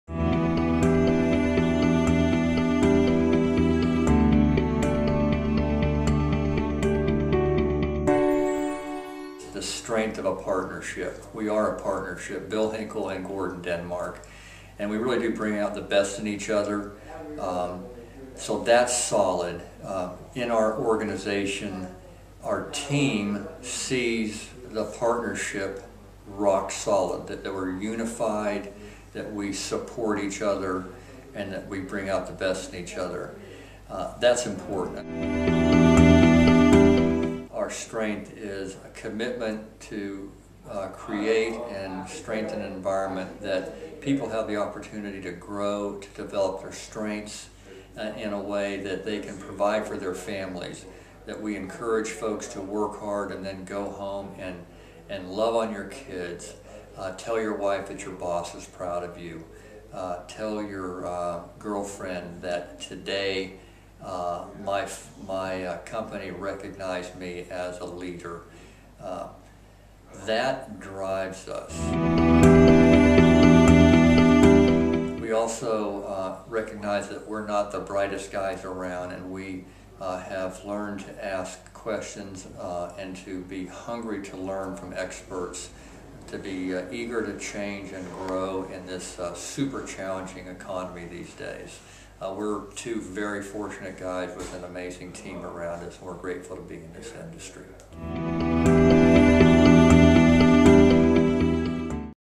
2012 Landscaper of the Year
Gordon Denmark, Landscaper of the Year co-winner with Bill Henkel from Henkel Denmark in Lexington, Kentucky, talks keys to success, what drives their business and how they've survived the recession.